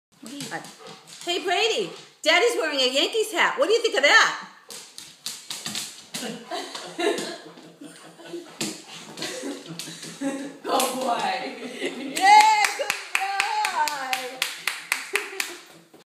Yankee Hater
Brady, the Golden Retriever, absolutely HATES those New York Yankees...